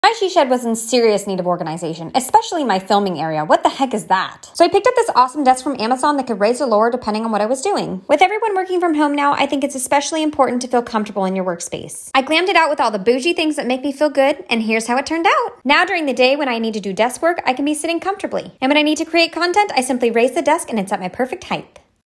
She Shed Makeover Mini Tour ~Amazon Must Haves~ Youtube Shorts
Working from home just got better! Check out this awesome adjustable desk I scored from Amazon!!! This desk is just what I needed!

Don't miss out on the Black Friday Sale Nov. 24 - 30th.
(This Item was Gifted)
(This video is meant to be viewed on you phone)